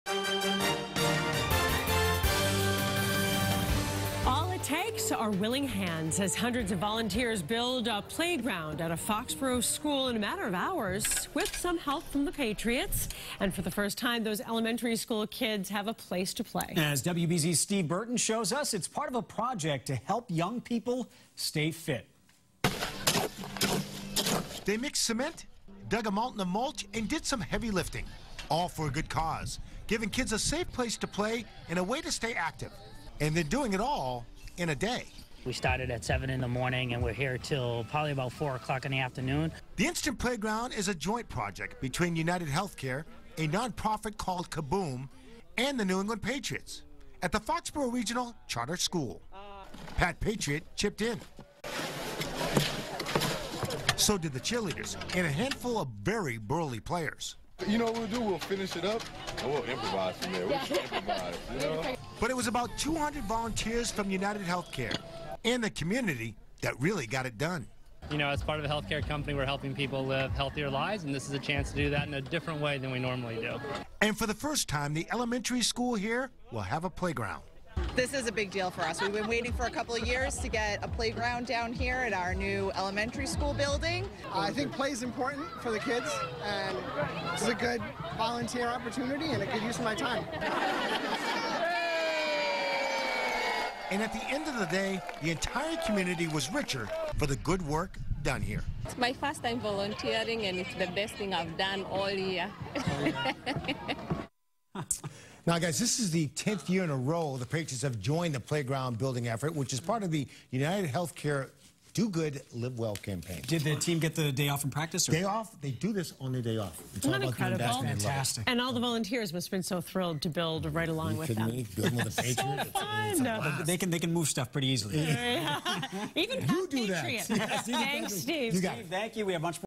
Instant Playground: New England Patriots Players Help Build a Playground
Some of the New England Patriots chipped in to build a playground for a charter school in Foxboro. WBZTV's Steve Burton has the story.